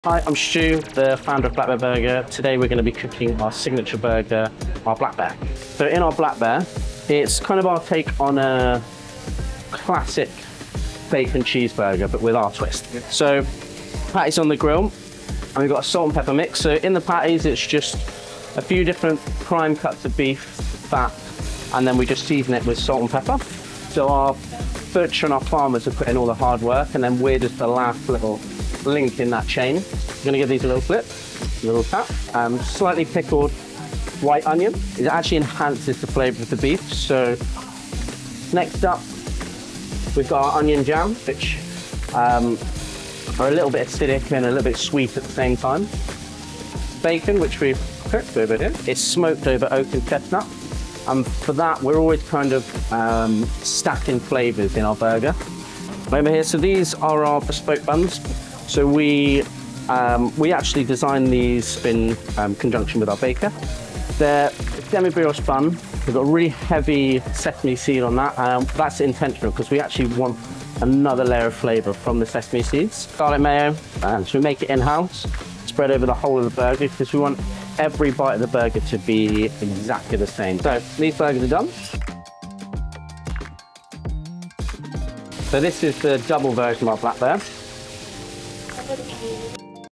London's Best Burgers - how we make our Black Bear Burger
We're know for having some of the best burgers in London and the UK. Here's how we make our signature burger, our Black Bear Burger